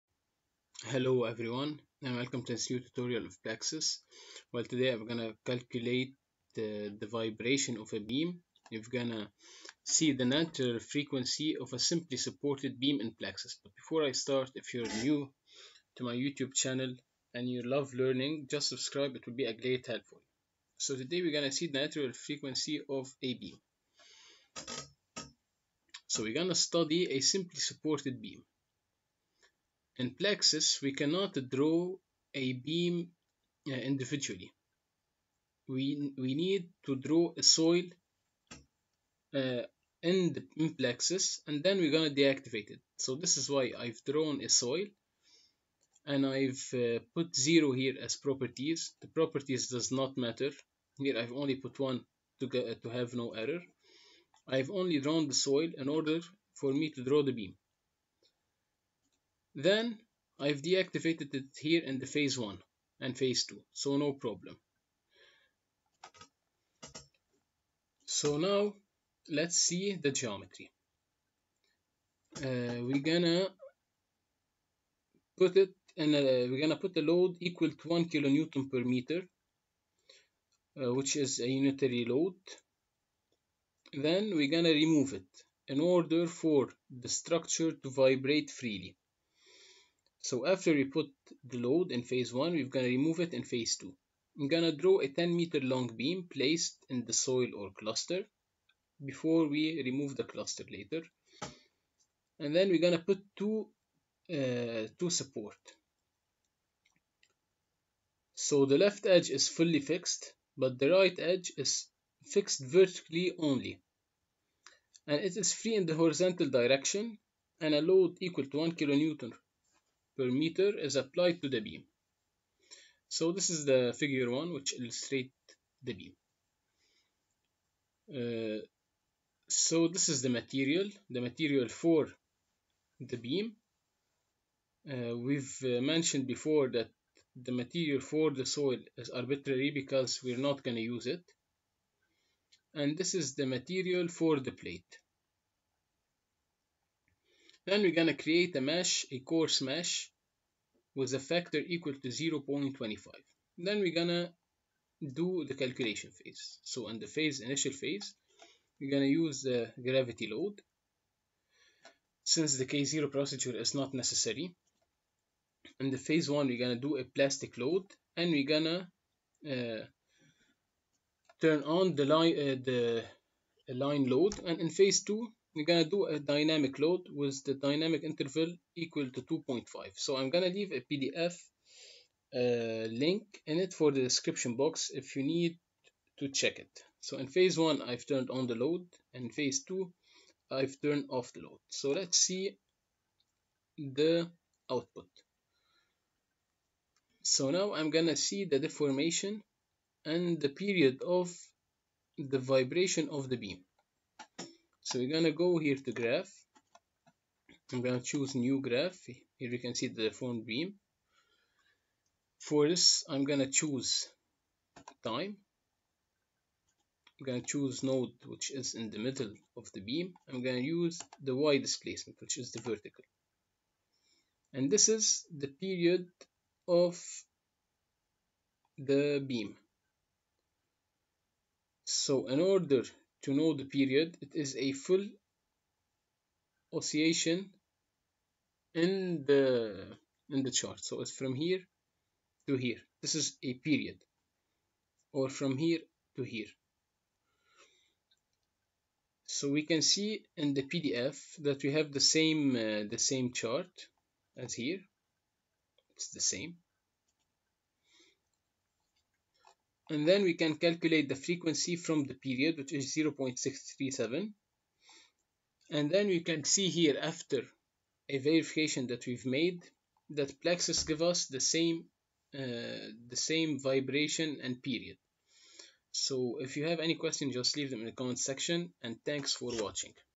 beam design on plaxis
easy beam design plaxis
calculation of natural frequency of beam on plaxis
structural beam design plaxis
structural design on plaxis

If you want to become the best engineer, you'r at the right channel!!